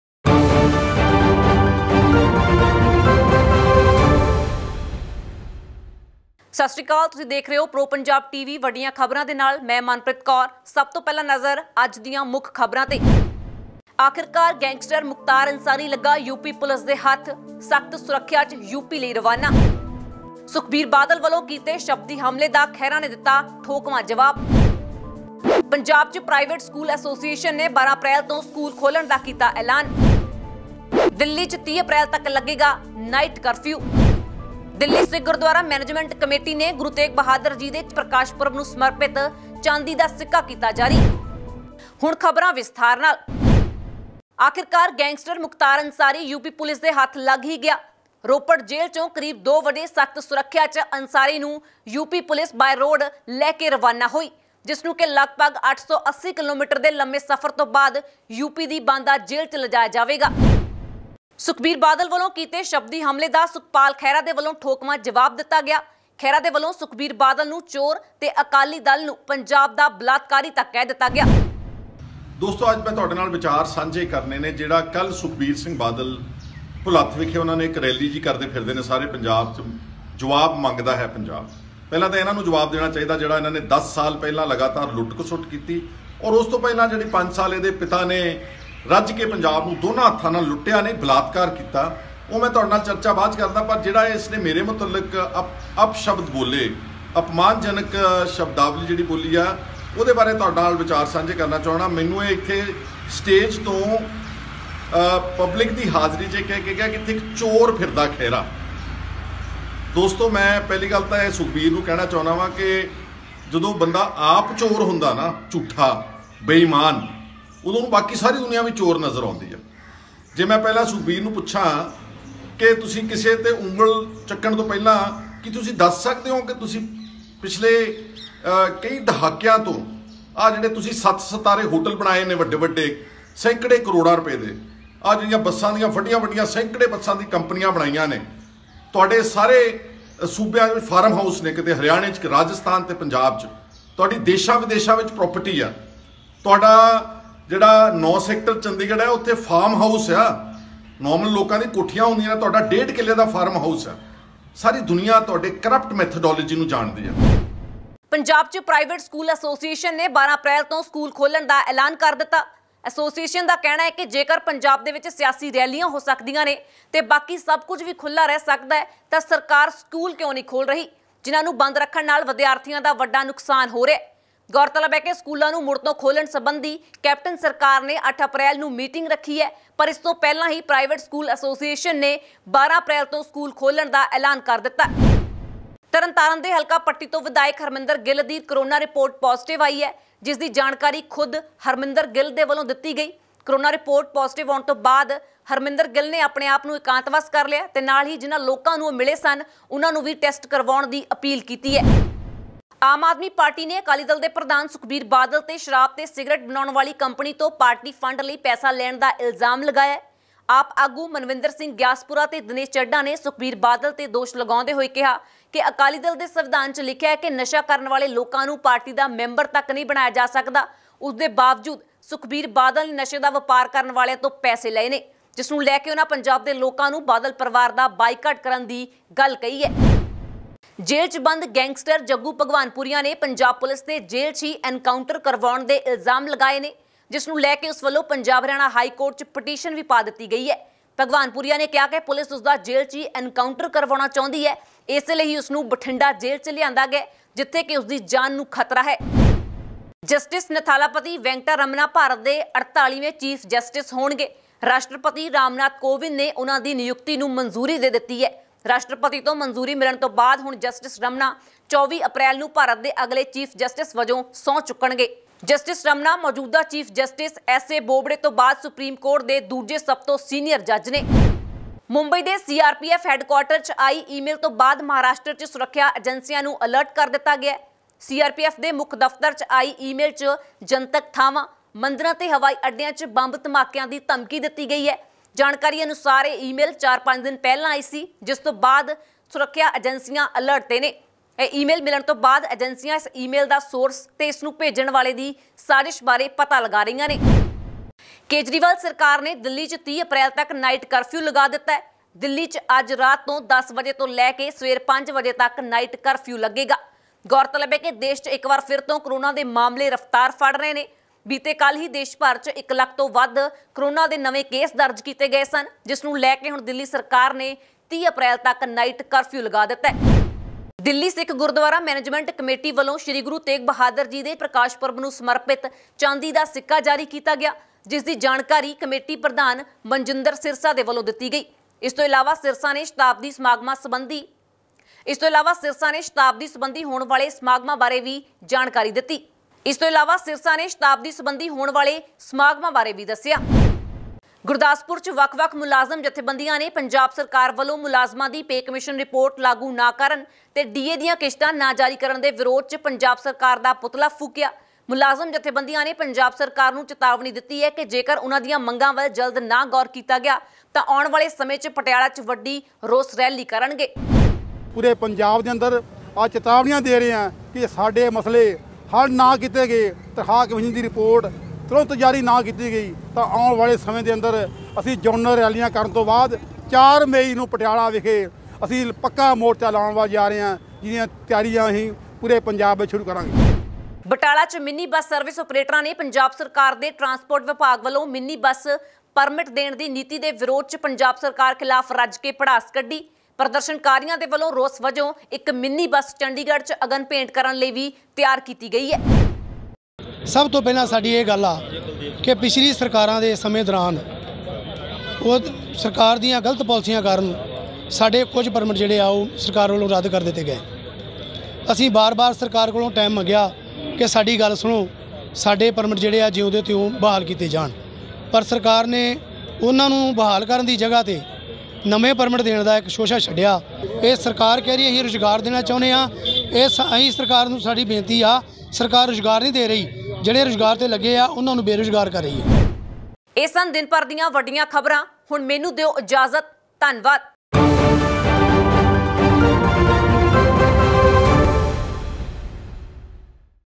ਆਖਿਰ ਅੰਸਾਰੀ ਲੱਗਾ UP ਪੁਲਿਸ ਦੇ ਹੱਥ,ਖਹਿਰਾ ਦਾ ਸੁਖਬੀਰ ਨੂੰ ਠੋਕਵਾਂ ਜਵਾਬ,ਦਿੱਲੀ ਚ 30 ਅਪ੍ਰੈਲ ਤੱਕ Night Curfew
ਆਖਿਰ ਅੰਸਾਰੀ ਲੱਗਾ UP ਪੁਲਿਸ ਦੇ ਹੱਥ, ਖਹਿਰਾ ਦਾ ਸੁਖਬੀਰ ਨੂੰ ਠੋਕਵਾਂ ਜਵਾਬ, ਦਿੱਲੀ 'ਚ 30 ਅਪ੍ਰੈਲ ਤੱਕ
Night Curfew
ਦੇਖੋ ਦਿਨ ਭਰ ਦੀਆਂ ਵੱਡੀਆਂ ਖ਼ਬਰਾਂ